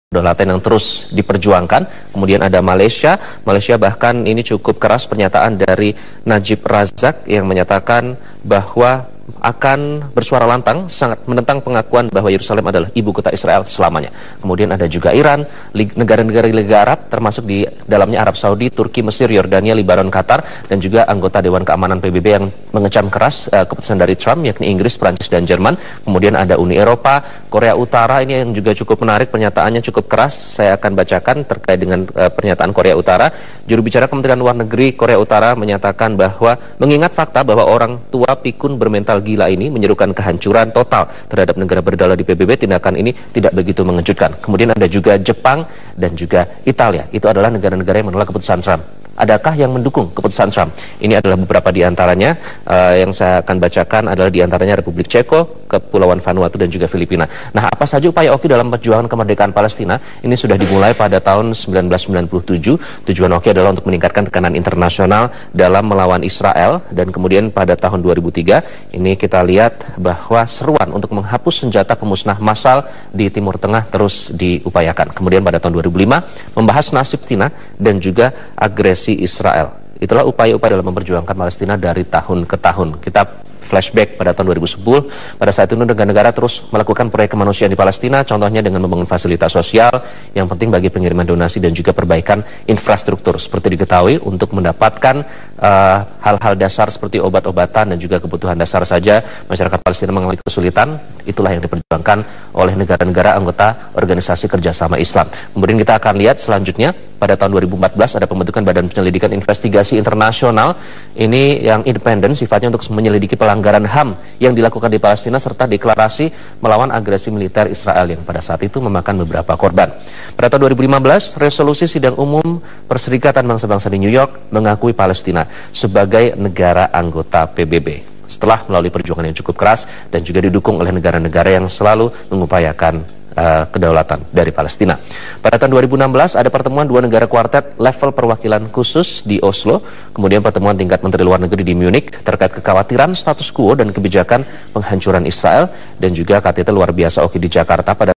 Indonesia diurutan pertama, negara yang menolak pernyataan Trumb
Kami segenap bangsa indonesia sangat mencintai kedamaian dan menjunjung tinggi HAM